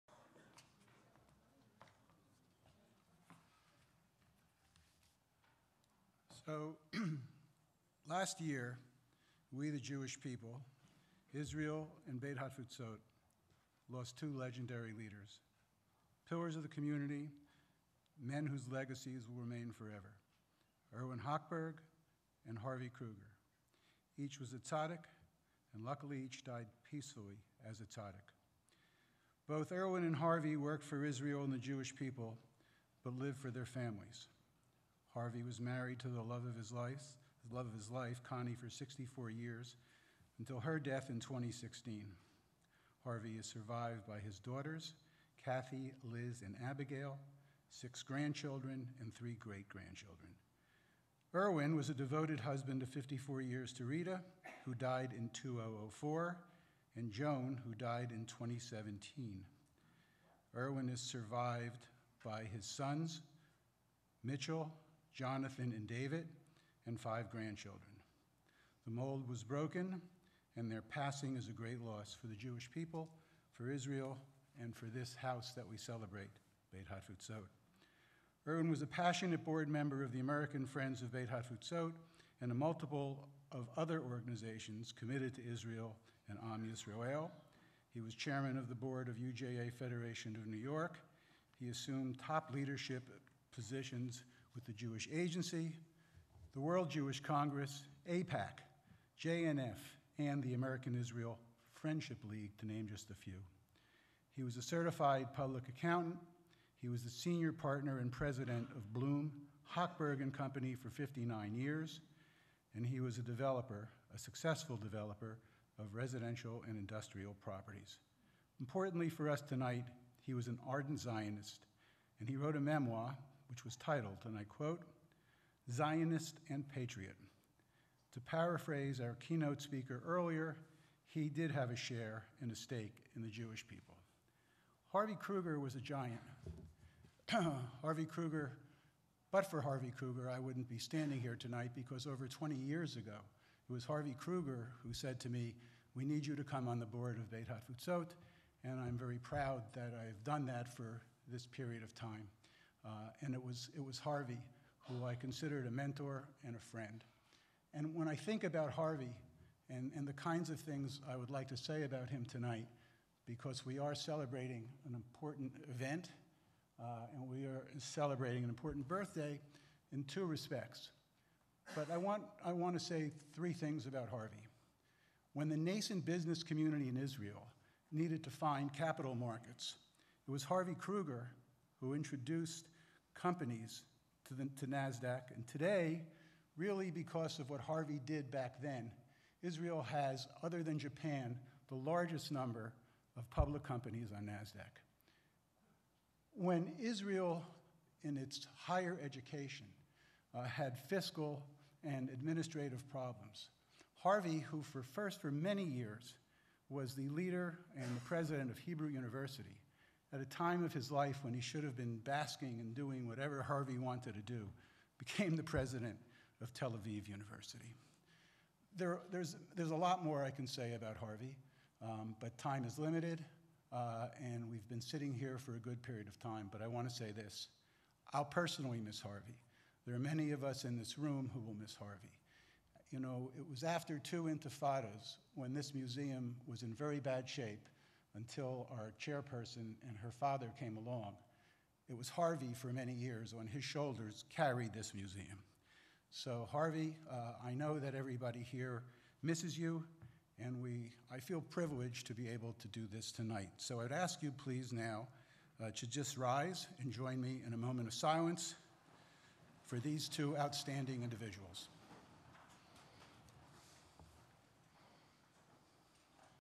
Stephen Greenberg, The Museum of the Jewish People at Beit Hafutsot
Stephen Greenberg pays tribute to Harvey Krueger and Irwin Hochberg. The Museum of the Jewish People at Beit Hafutsot